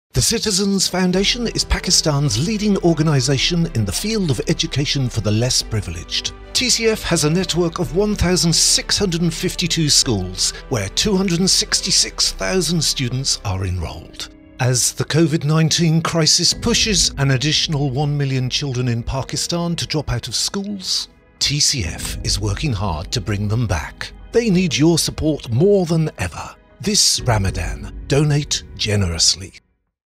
Help Transform Lives Through Education | TCF (UK)
TCF has a network of 1,652 schools in the heart of urban slums and rural communities across Pakistan where 266,000 students are enrolled.

As the Covid-19 crisis pushes an additional 1 million children in Pakistan to drop out of schools, TCF is working hard to bring them back.

They need your support more than ever! This Ramadan, Donate Generously!